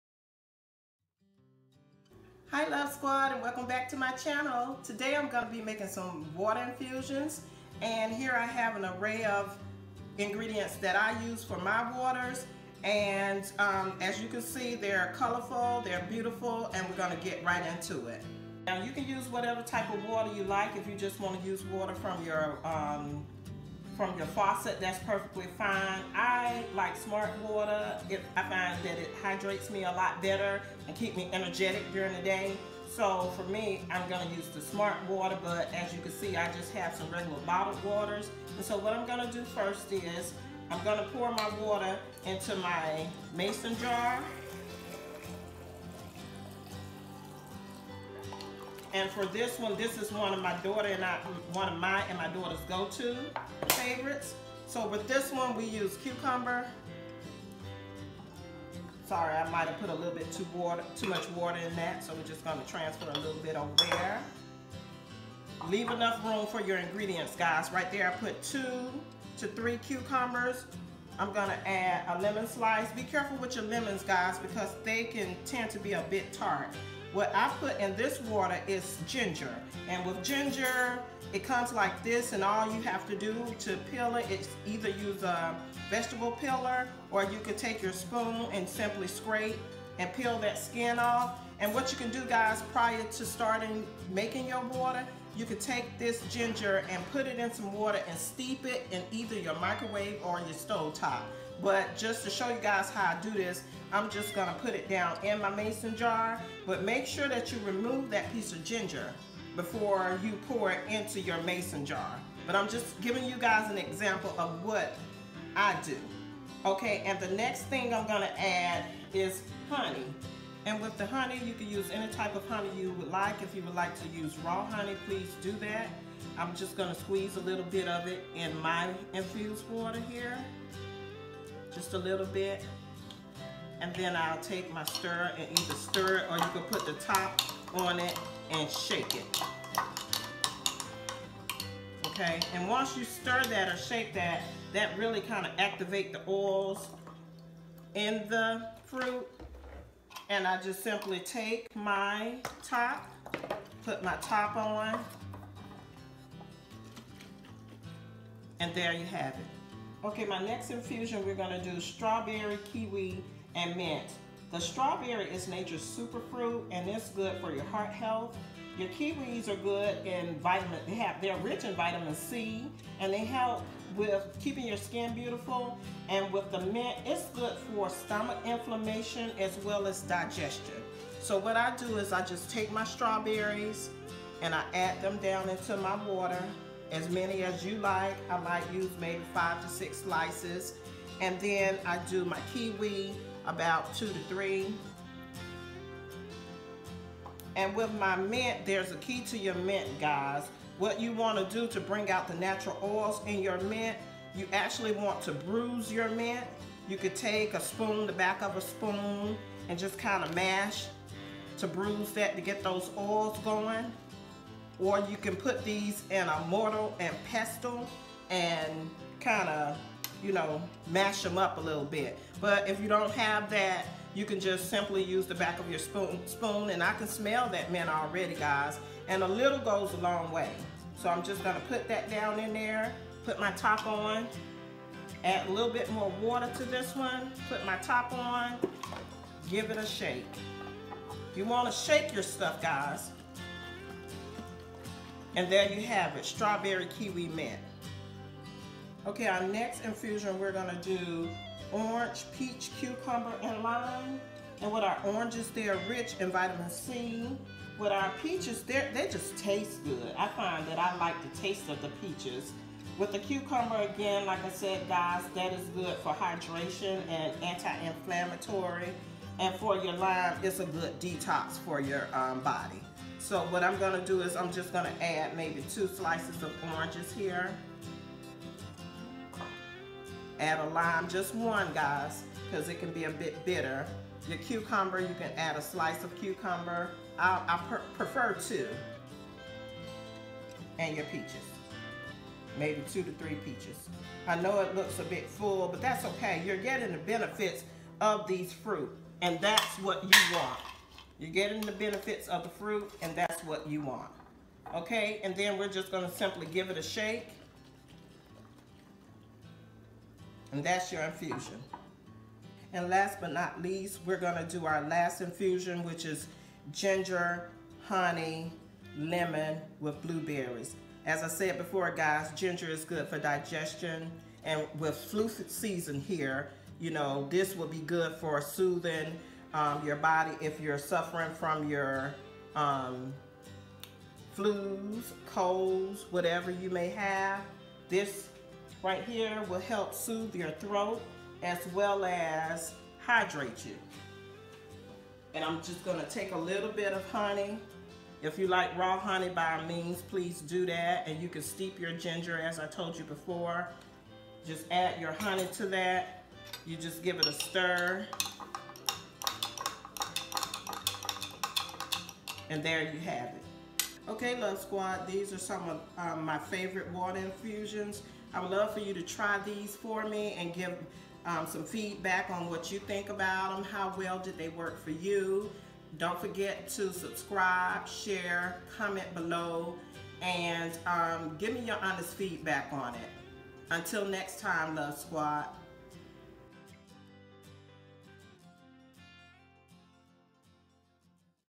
Energizing Infused Water
Revitalizing and refreshing waters to keep you going throughout the day